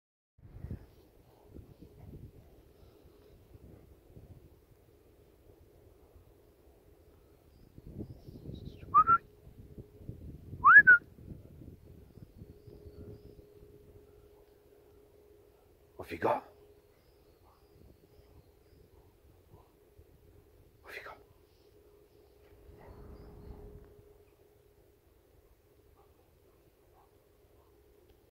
Rottweiler gets excited after finding old ball !!
Bane finds one of his lost balls on a walk and is so excited and proud of himself!